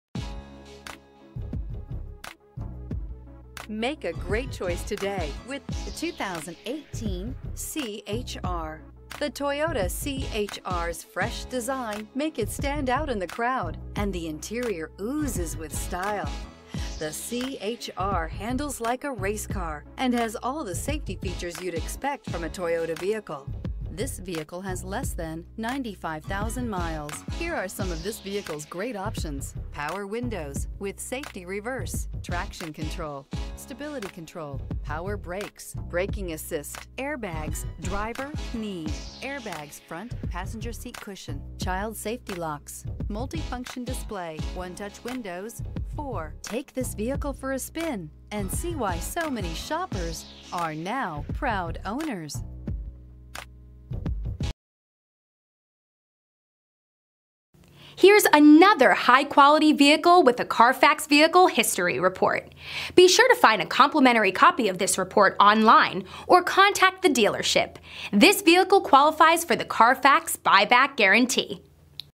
2018 Toyota C-HR Houston TX NA11674B
2018 Toyota C-HR XLE

This vehicle is a 2018 Toyota C-HR XLE with a 2.0 liter 4 cylinder engine and sequential port fuel injection. It has an impressive fuel economy of 27 mpg in the city and 31 mpg on the highway, as well as a 5 out of 5 stars crash test rating for safety. The black interior adds to its aesthetic appeal, while hill start assist provides additional convenience features for drivers. This car has driven 90,004 miles.